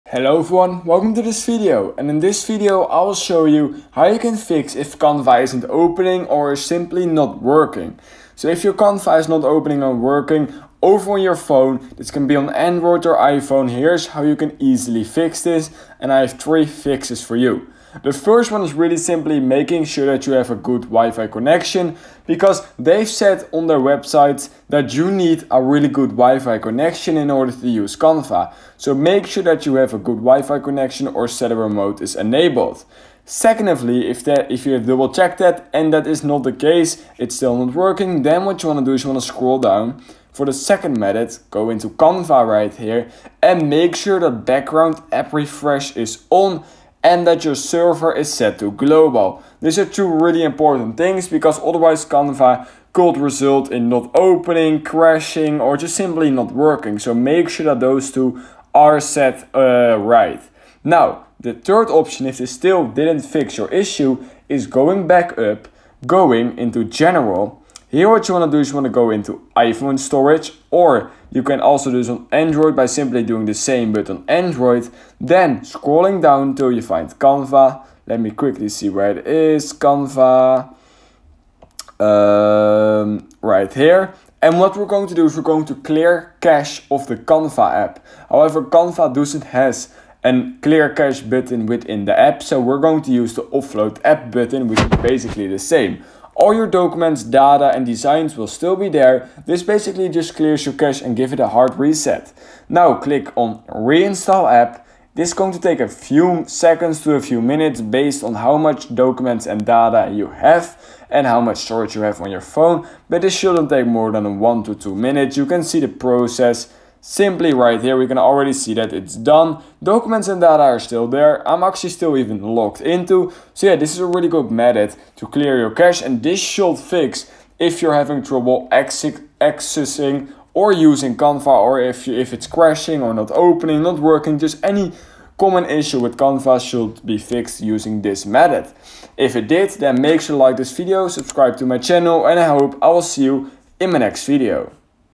How To Fix Canva App Not Opening Mobile
How To Fix Canva App Not Opening Mobile - Full Tutorial

👉 In this video we are going to show you how you can fix if the Canva app isn't opening on mobile.